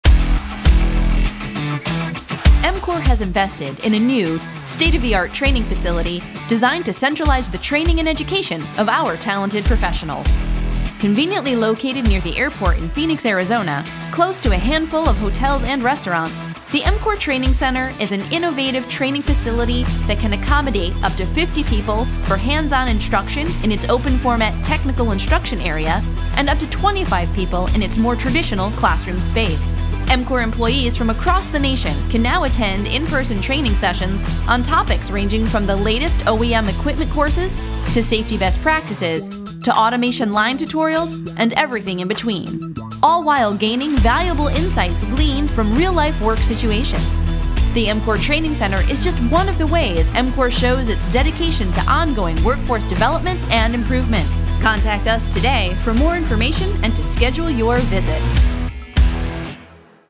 EMCOR’s Cutting-edge Training Center Shows Our Dedication to Workforce Development
EMCOR has invested in a new state-of-the-art training facility designed to centralize the training and education of our talented professionals.

Equipped with both hands-on instruction areas and traditional classrooms, the center offers courses on everything from the latest OEM equipment to safety best practices to automation line tutorials and everything in between.